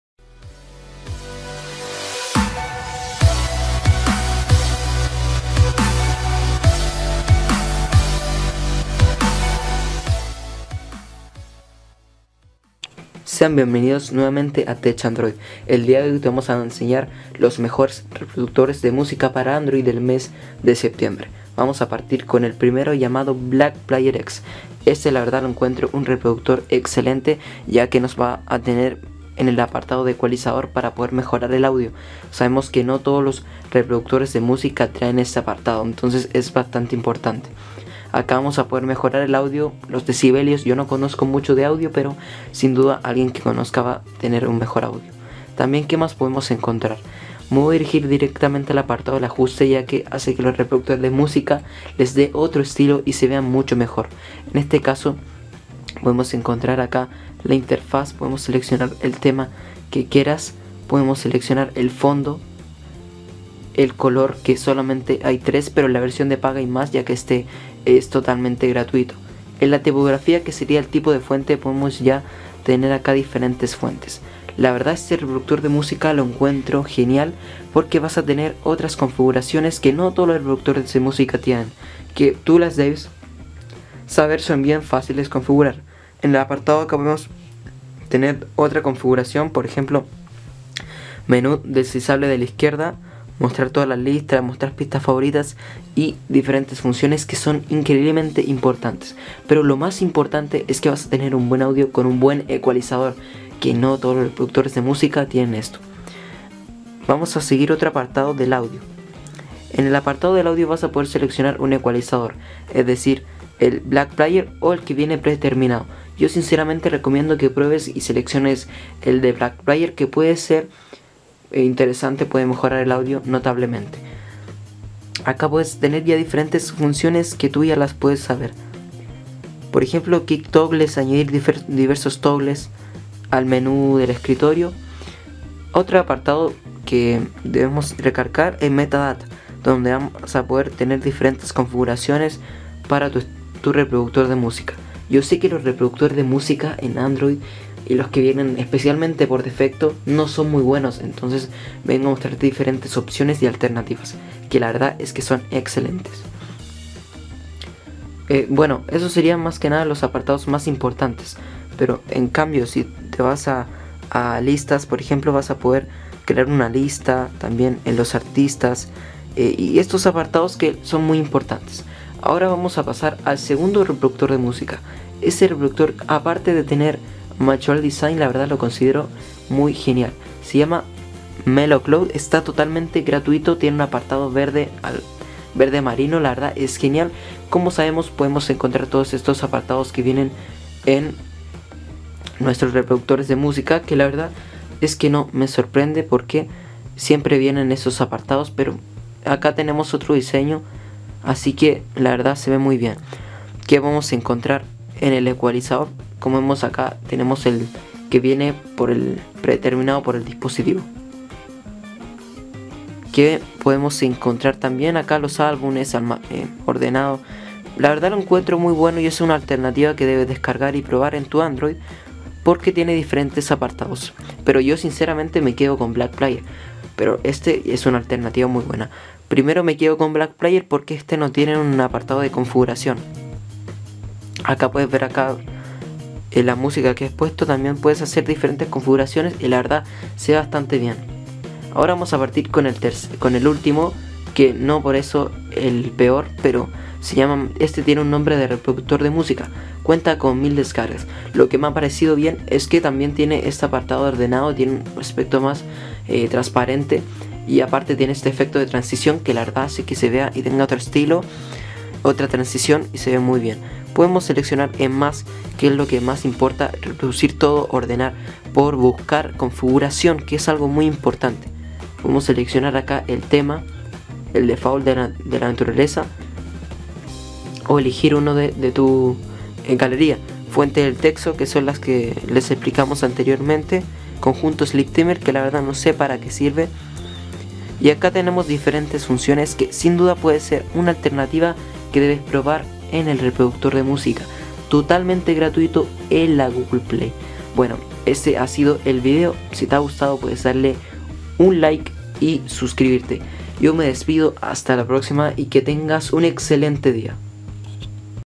Los Mejores Reproductores de musica android 2015 - Excelentes Ecualizadores Septiembre
Lista de reproductores gratis en google play